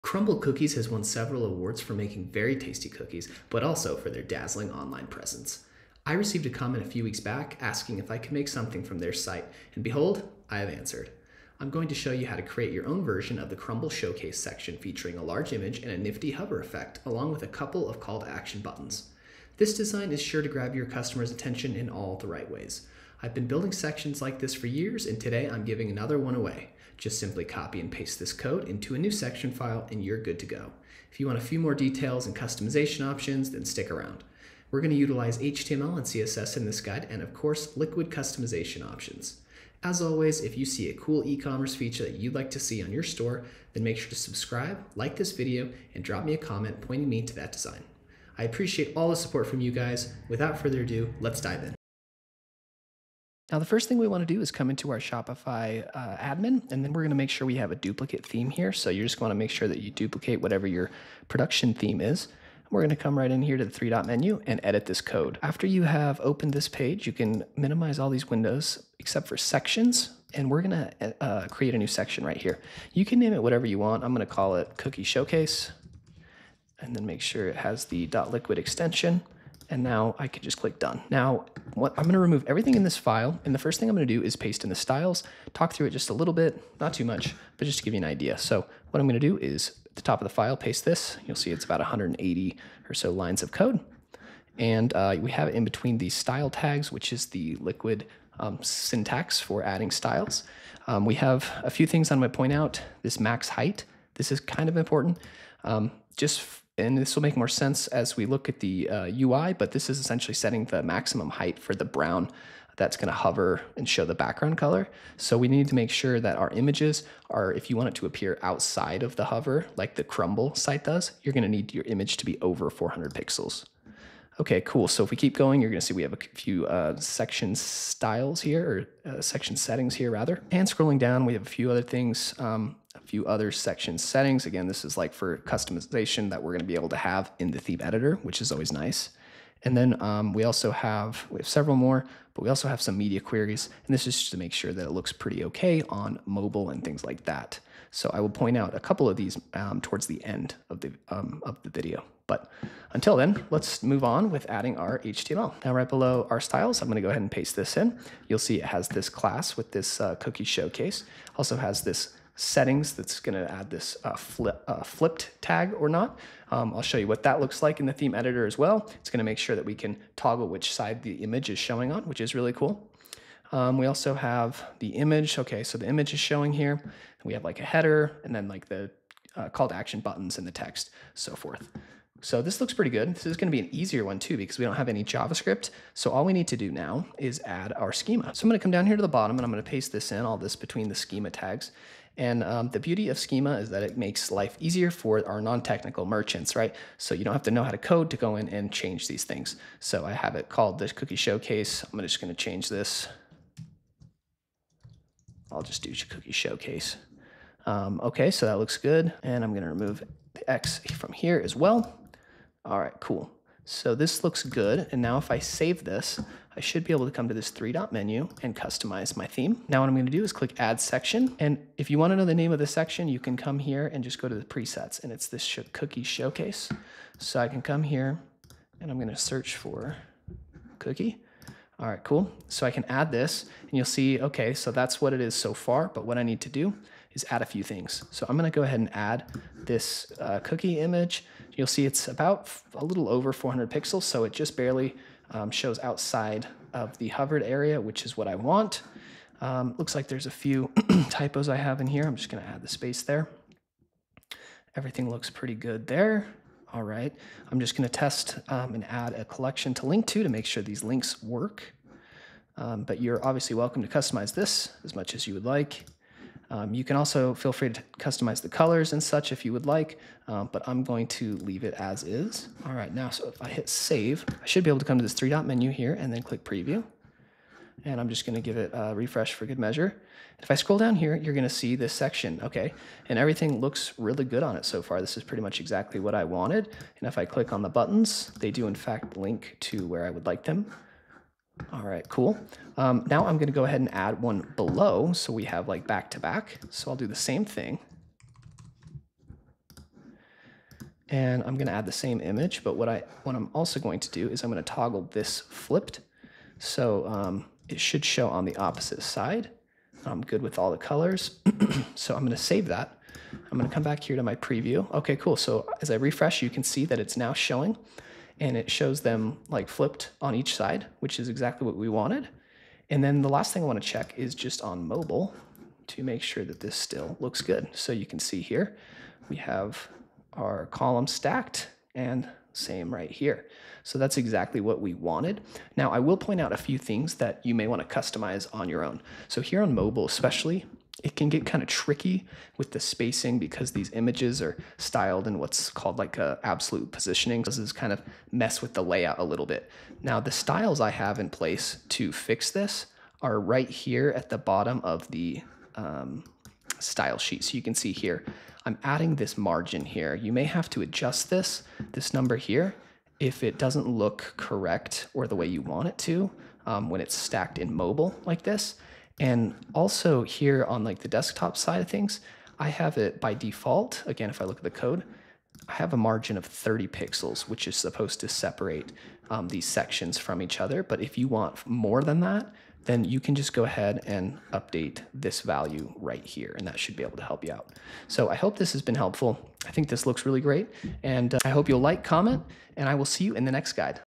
Product Showcase Inspired by Crumbl (Copy/Paste)
If you're looking to enhance your Shopify store with a visually appealing section that showcases your products, you're in the right place! Inspired by the Crumbl Cookies website, we’ll guide you through adding a stylish product showcase section to your store. This section will feature images, titles, descriptions, and buttons that encourage customers to engage with your products.